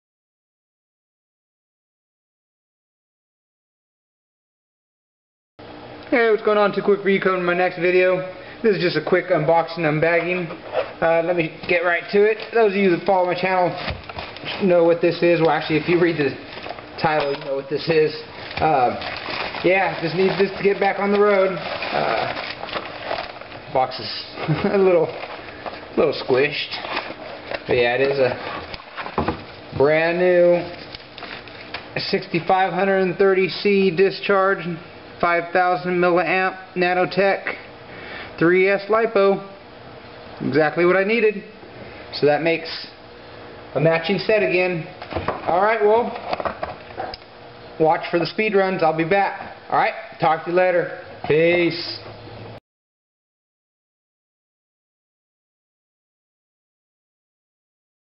unboxing speed run battery. 2cwik4u #251
got my new turnigy nano-tech 3s 65c-130c lipo to replace the one I destroyed. these are awesome batteries. cant beat the price for the punch.